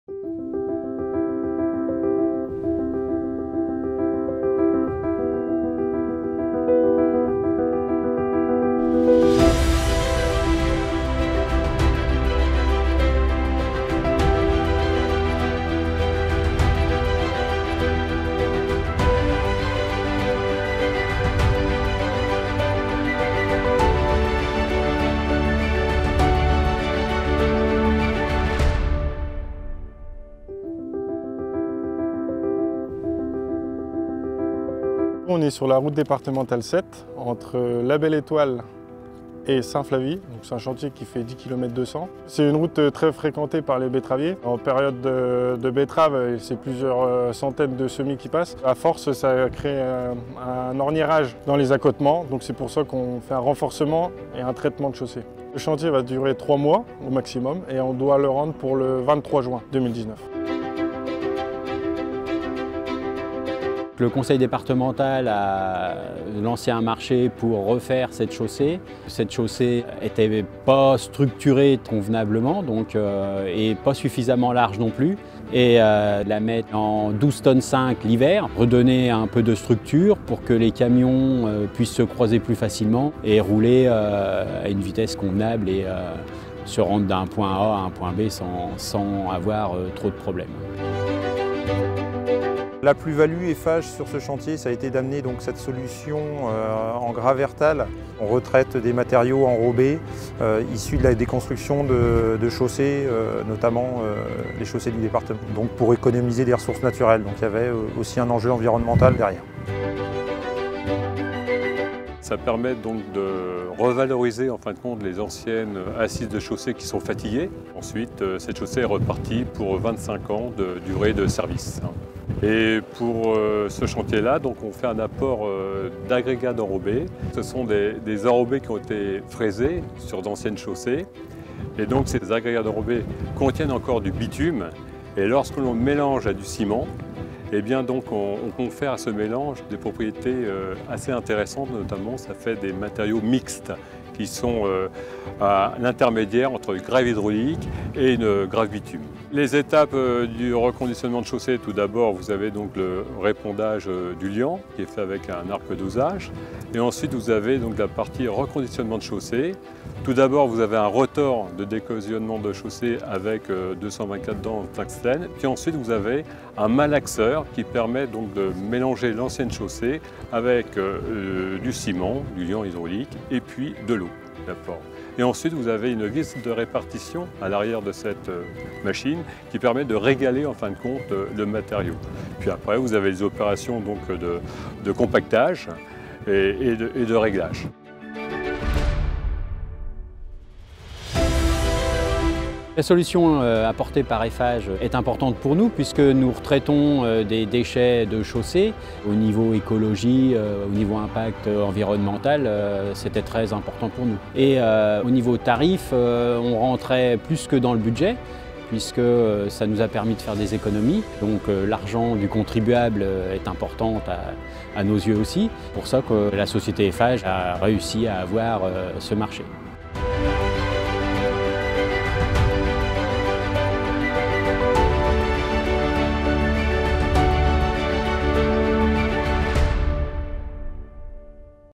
[Chantier] Réfection de la RD7 - Eiffage Route
EiffageRoute présente le chantier de réfection de la RD7 pour le conseil départemental de l'Aube.